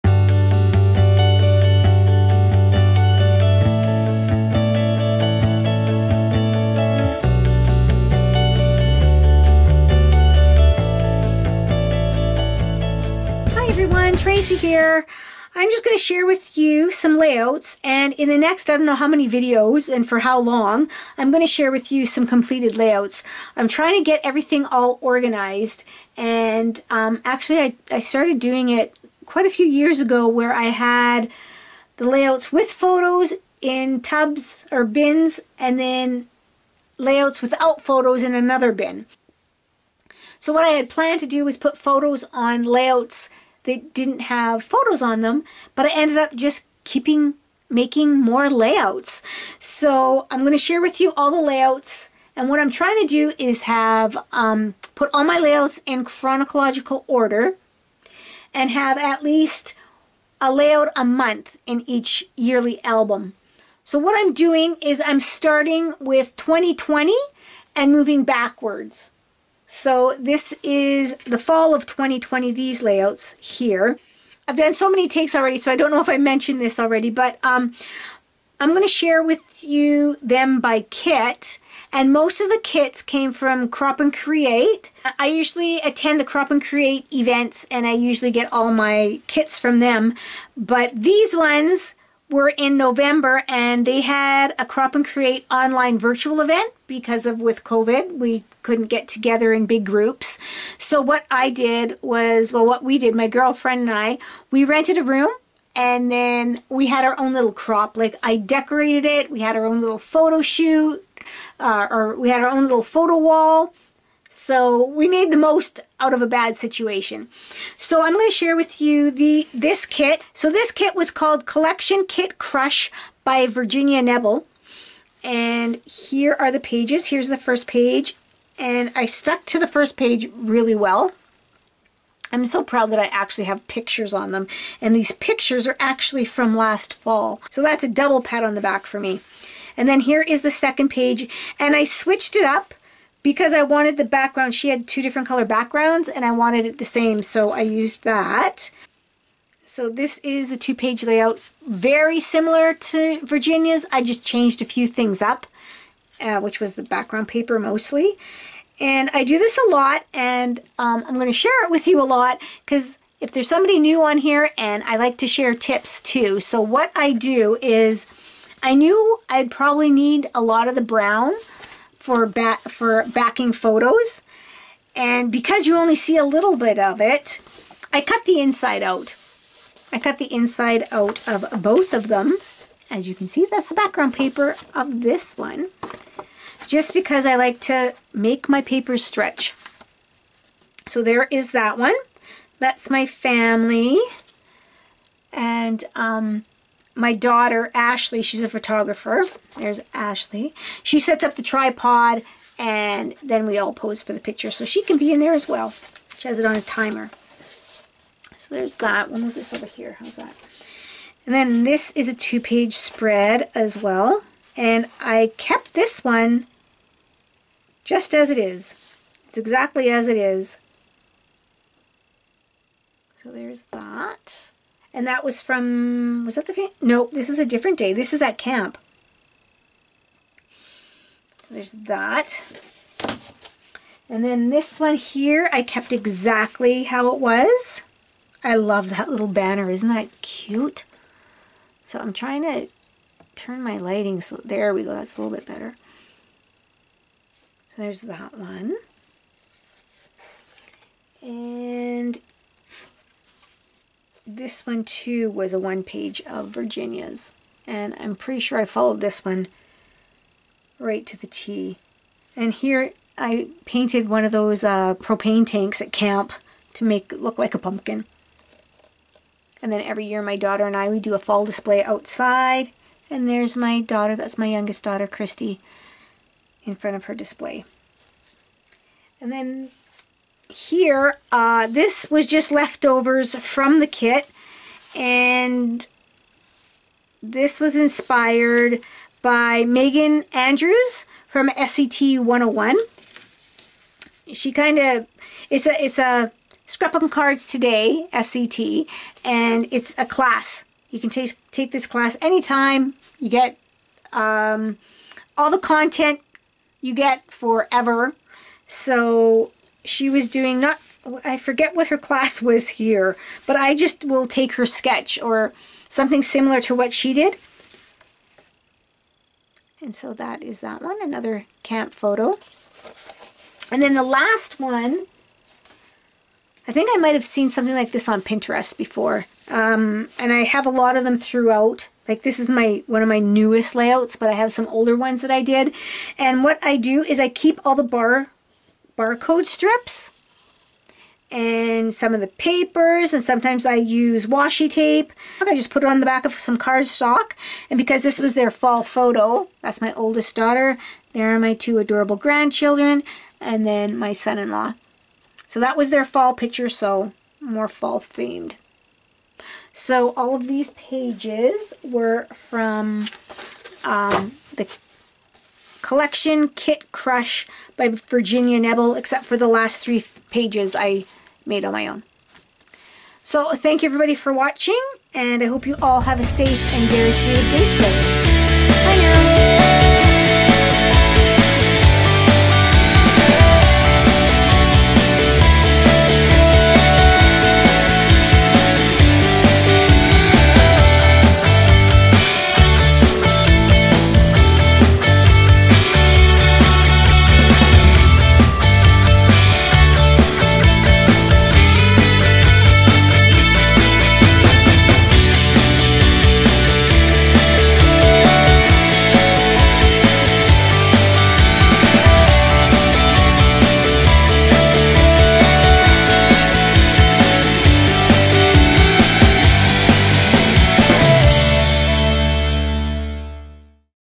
Collection Crush Layouts
Here are the layouts I created with the Collection Crush Kit. The first 4 layouts were designed by Virginia Nebel. The last 2 were designed by me :)

Come and visit me.